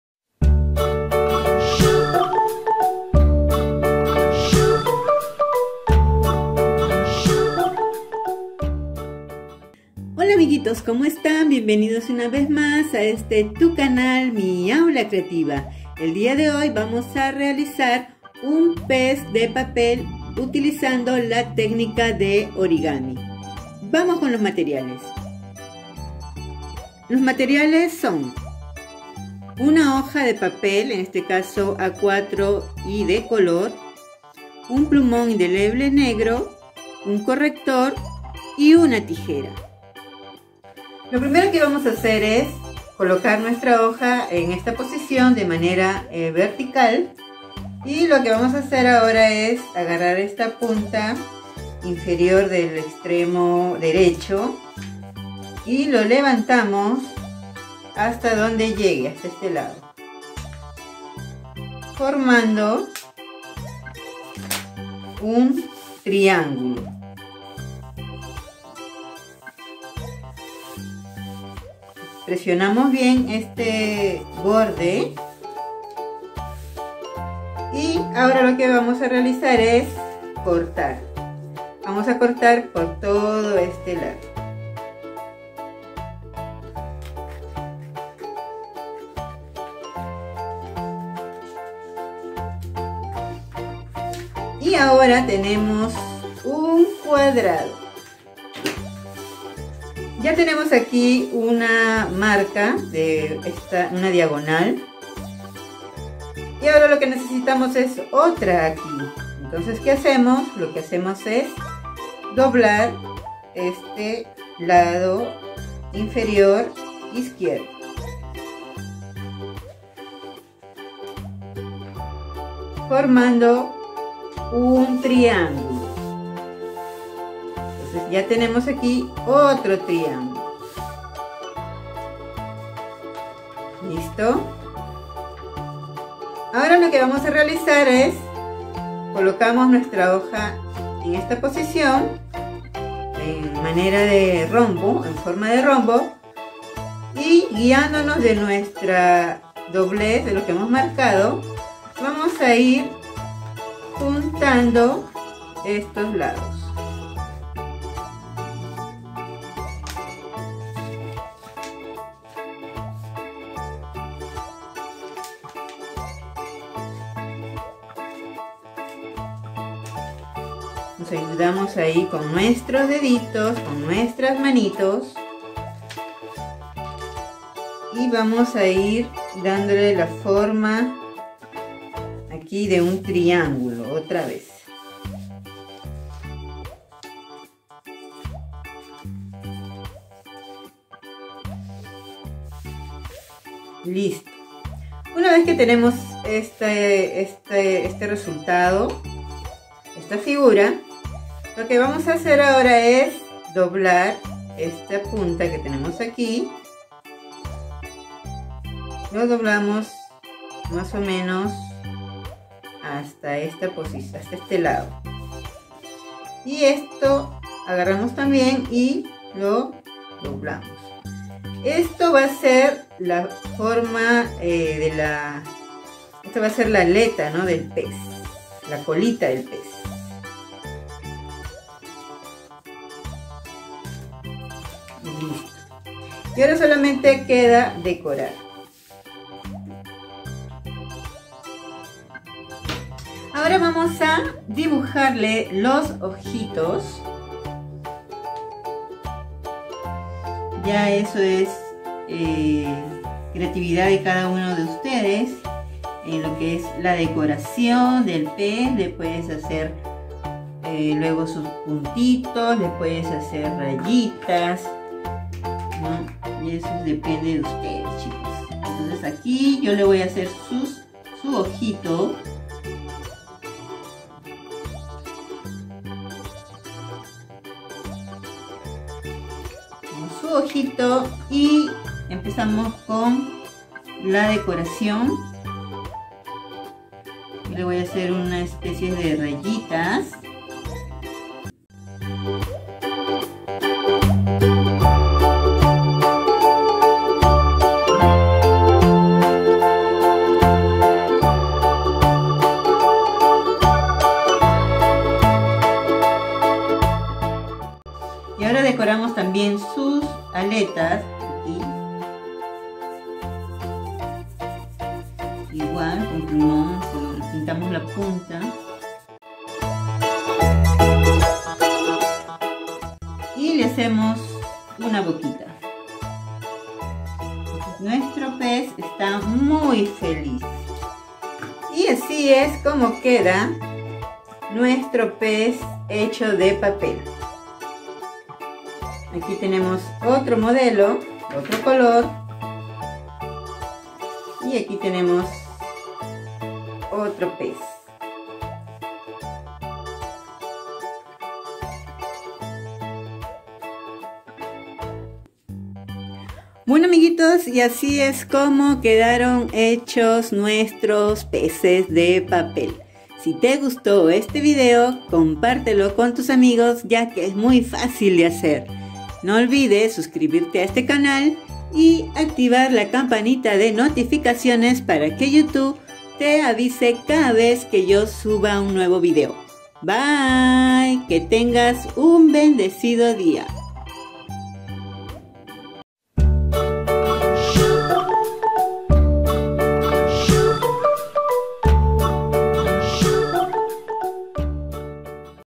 TECNICA ORIGAMI | HAGAMOS UN PEZ DE PAPEL | MUY FÁCIL
Hola chicos y chicas, hoy tenemos una técnica de origami donde haremos un Pez de Papel.. de una manera fácil y divertida... quieres saber como hacerlo... vamos a ver el video.